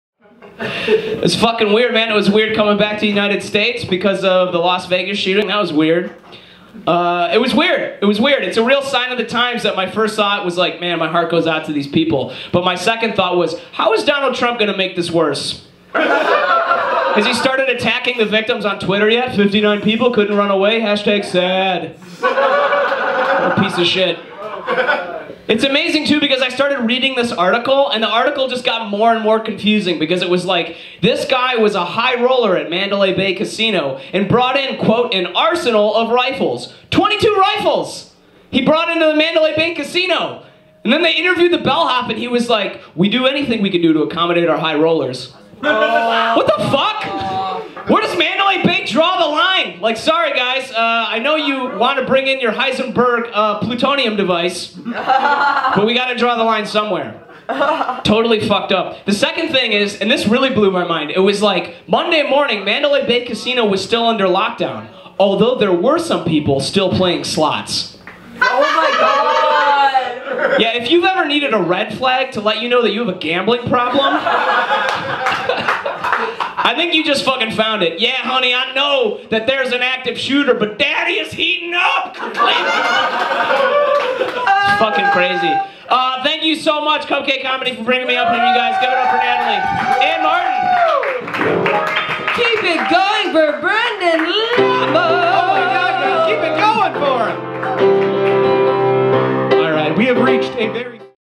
Brendon Lemon, Stand Up Comedy: Vegas Shooting
At Gallery Cabaret, Chicago, IL for Cupcake Comedy